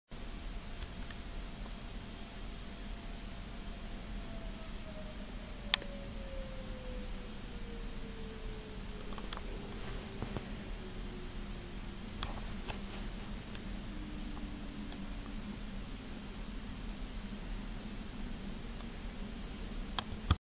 Dravosburg STH-10 activation, Missed most of it
Yep, My shelves were once again a problem on getting to the camcorder.